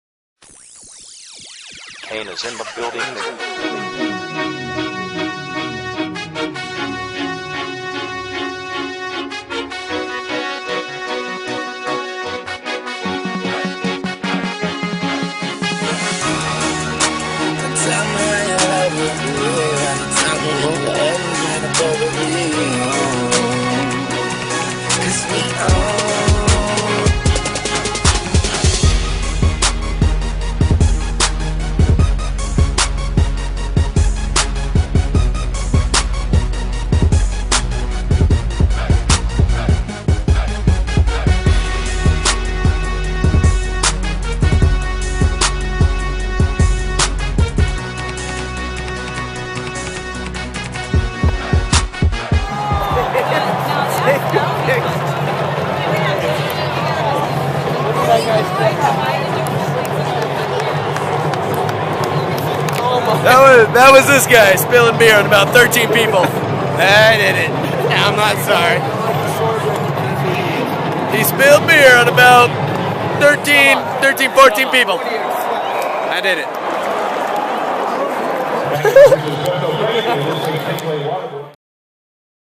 Broncos vs Packers 2011
Me and Bro went to Lambeau field and it was quite an amazing time. The people were classy and humble hosts. Except for a few, but thats to be expected anywhere at an opposing NFL stadium.